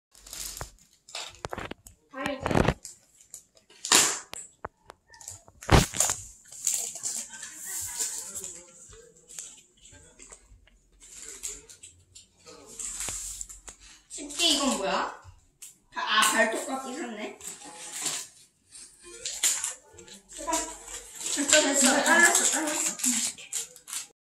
생선뼈 장난감을 무서워하는 고양이 로나
생선뼈 모양의 장난감을 가지고 놀라고 줬는데, 뭔가 무서운지 뒷걸음질을 치는 아기고양이 로나입니다 ^^;;